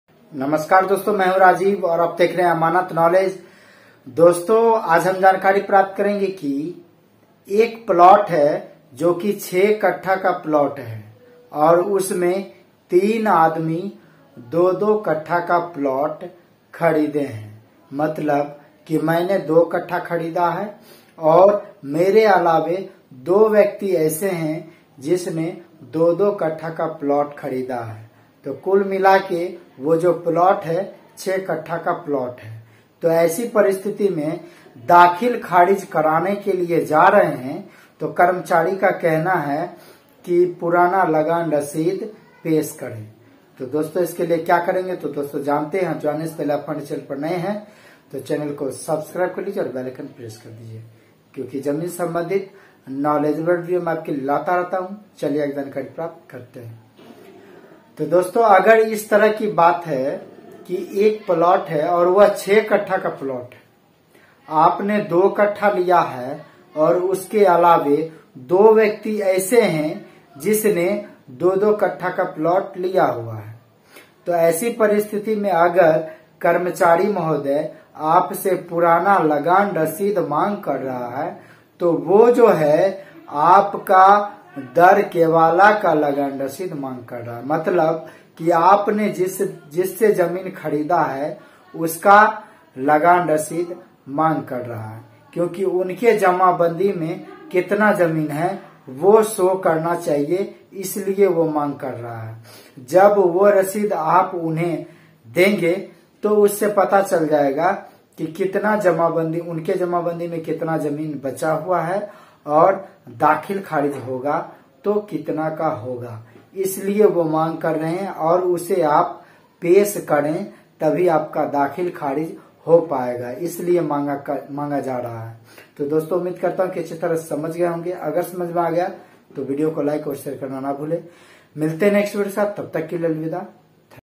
3 आदमी मिलकर 6 कट्ठा जमीन खरीदे हैं जिसमें मेरा 2 कट्ठा है कर्मचारी पुराना लगान रशीद माँग रहा है।
Namaskar dosto ye video isliye banaye hain taki aap ko ye jankari mil sake ki 3 aadmi milkar 6 kattha jamin kharide hain jisme mera 2 kattha hai karmchari purana lagaan rashid mang raha hai
ummid karta hoon ki ye video aap ko pasand aayega

MY GEARS:-